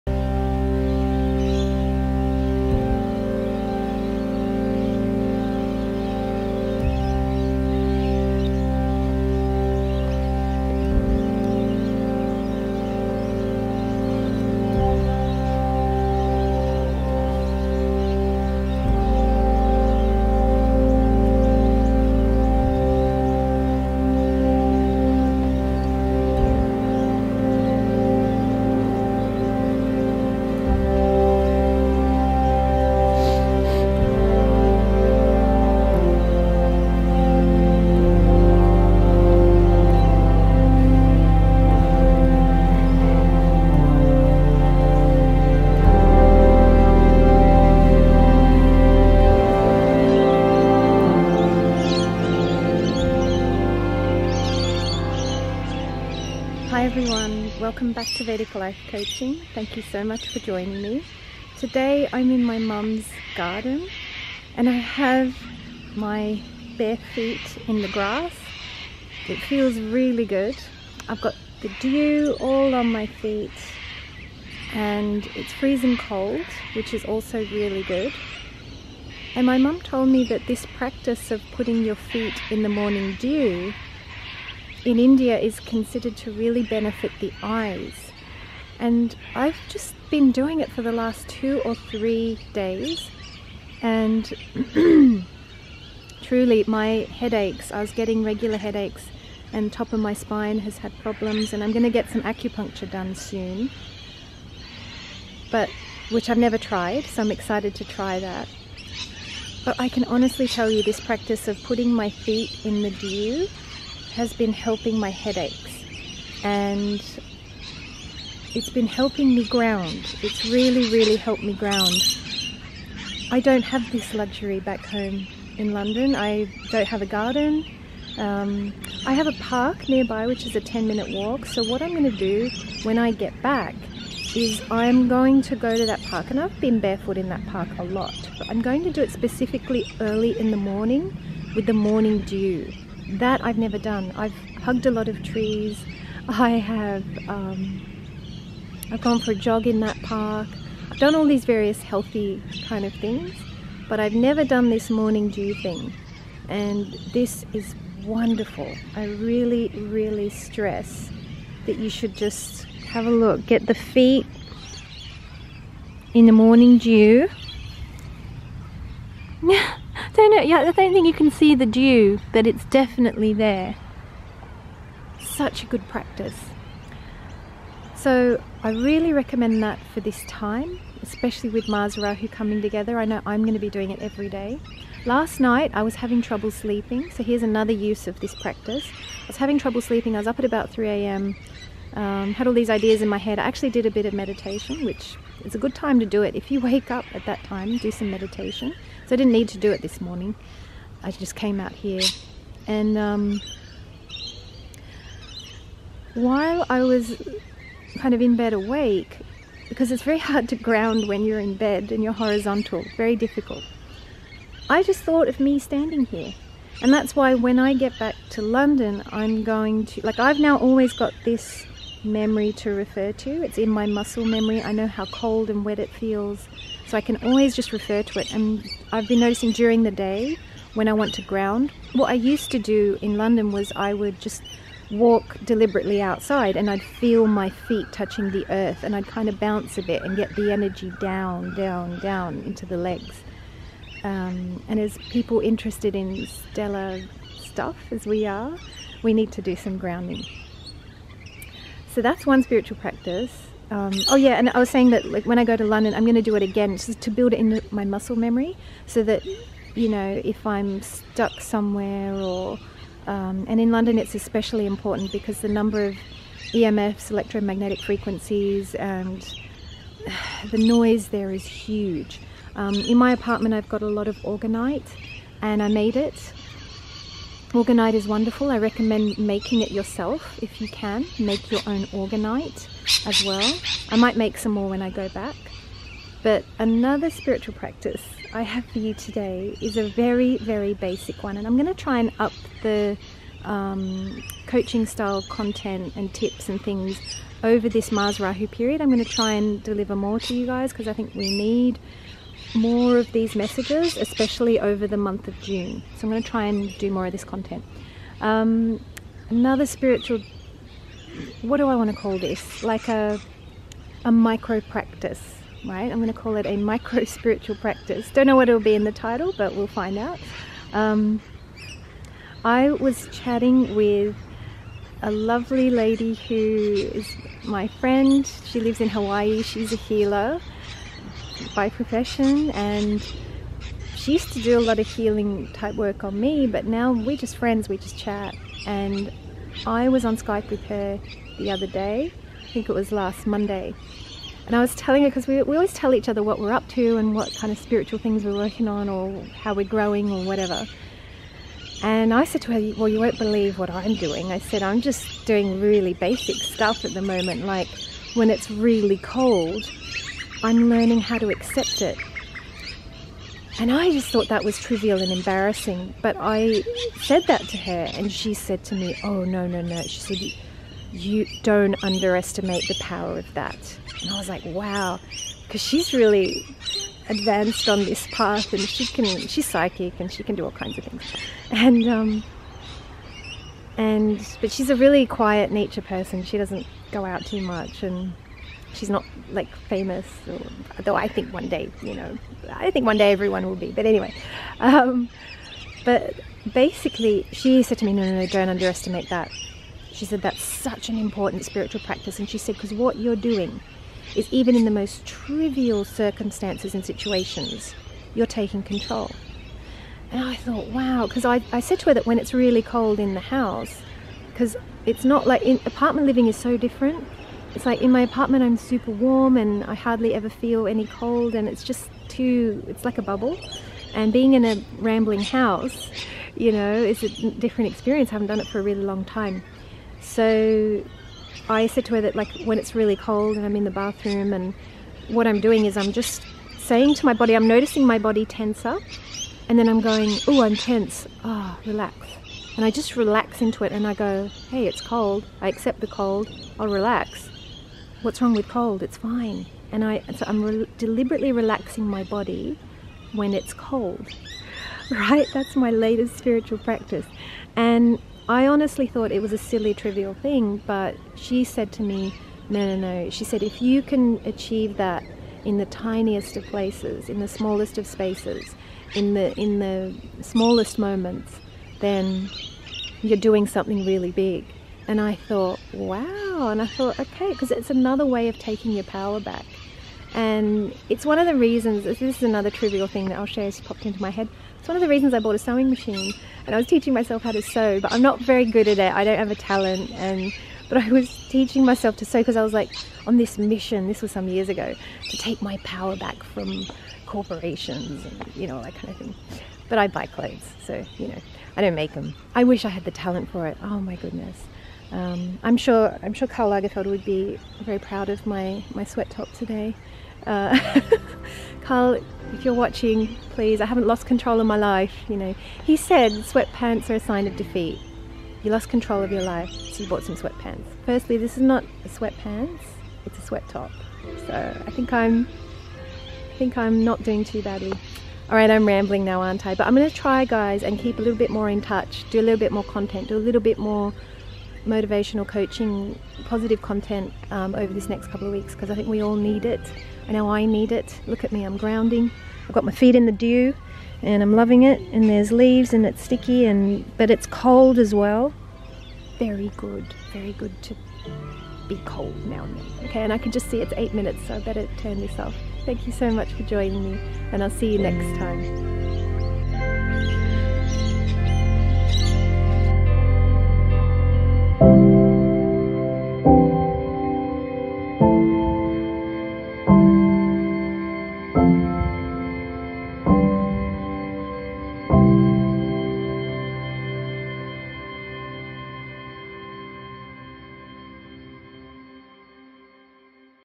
Morning dew walk + micro-spiritual practices
We’re all going to need more energy in the month of June – which is why I put this video together. I’m finding that a good 5-10 minutes of standing in the morning dew to be very helpful – I can do this for the next few months while I am at home in Sydney. However, if, like me, you live in an apartment – what are some natural ways to get a quick power boost?

Well, in my concept of micro-spiritual practices, you can create your own power boost just by observing your physical body and working out where you are spending energy unnecessarily. So, recently, I discovered that I tense up in the morning when it’s cold in the bathroom. Now, I relax into that cold feeling, which saves my energy and helps keep me relaxed all day. I'm getting better at spotting when I'm tense and then deliberately relaxing – it's good for reducing all kinds of anxiety and tension. Share your micro-spiritual practices in the comments below if you feel so inspired :)).

VIDEO CREDITS